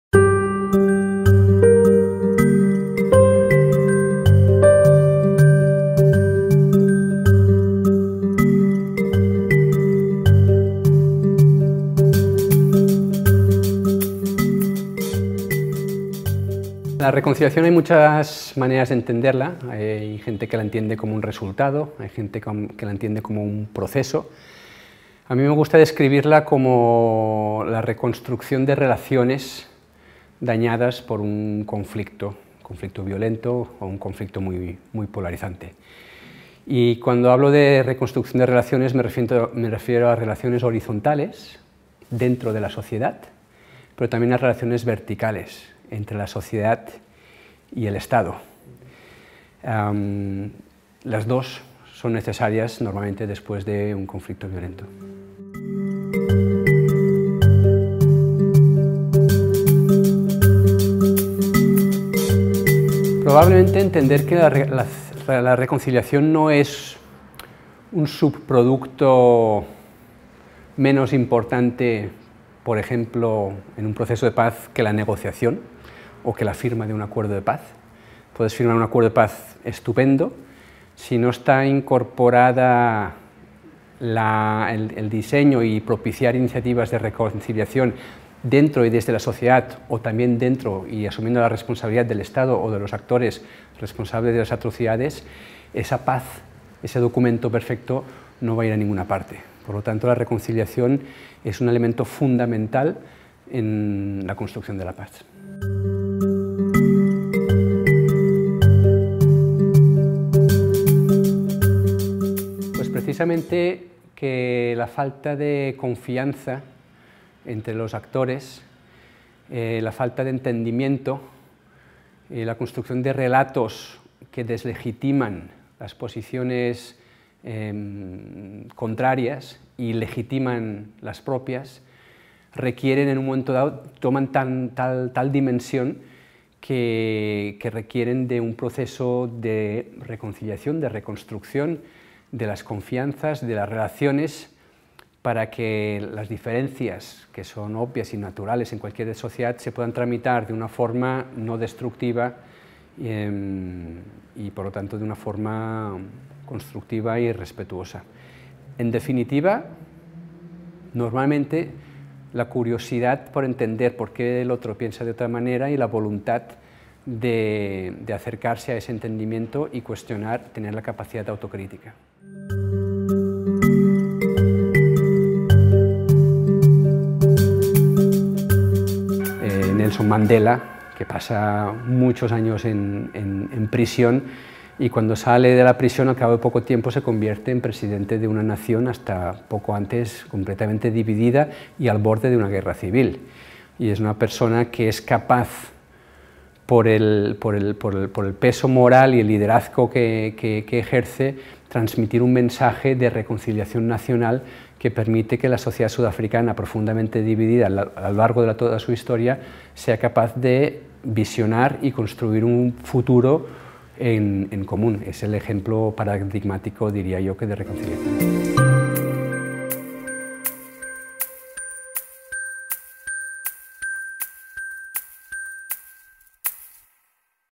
Entrevista con Kristian Herbolzheimer - Reconciliación en sociedades fracturadas
Entrevista con Kristian Herbolzheimer en el marco del curso "Fundamentos para una cultura de la reconciliación".